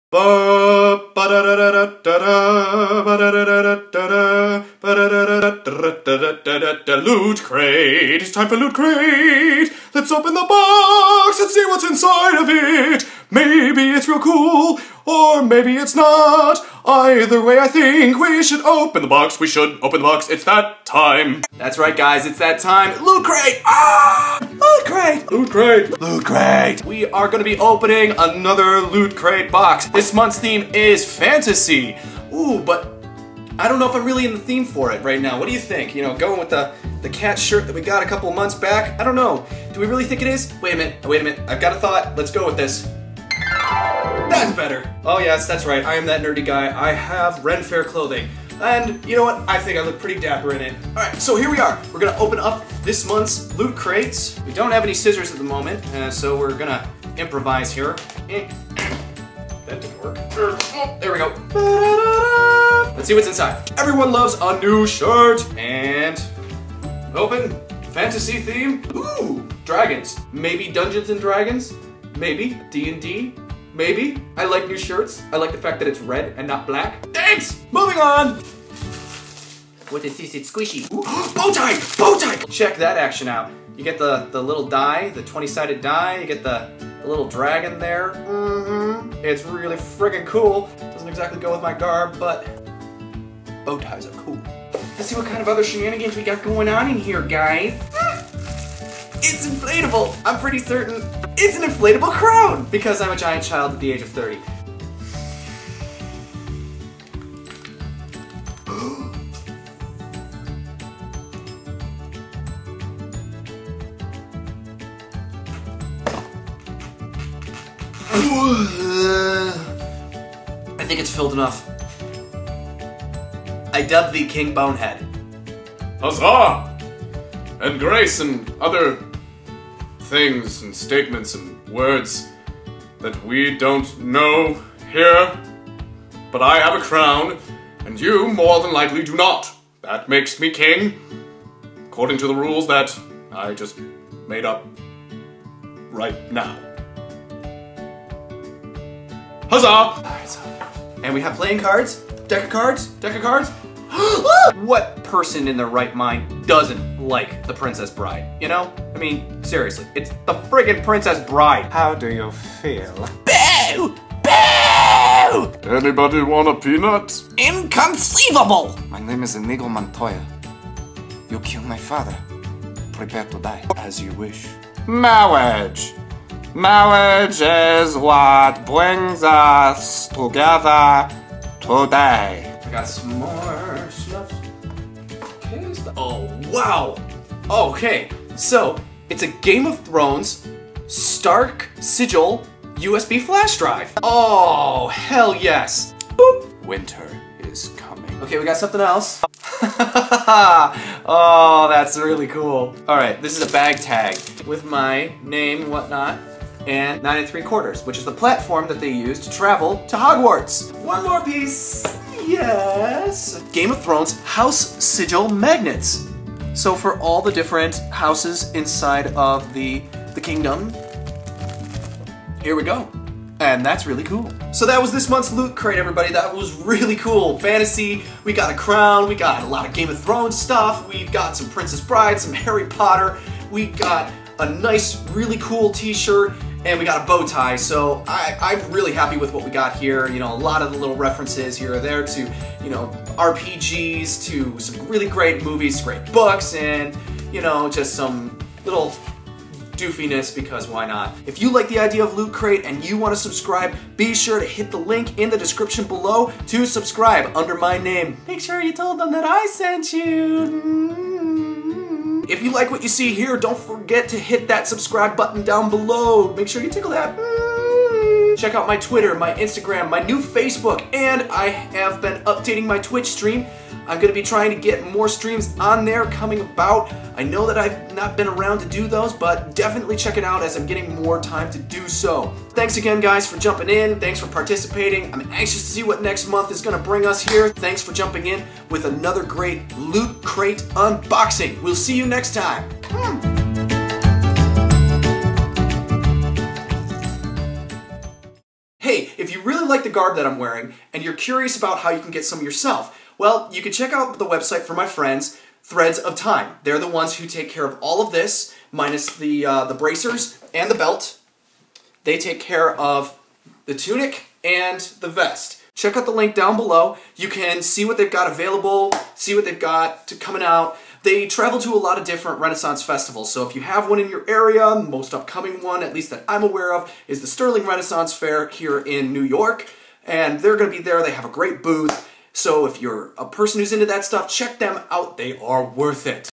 Loot Crate Unboxing - FANTASY
Enter a land of myth and imagination. It's this month's Loot Crate!! This time the theme is FANTASY!!

Don't you love my threads?? Then check out Threads of Time!!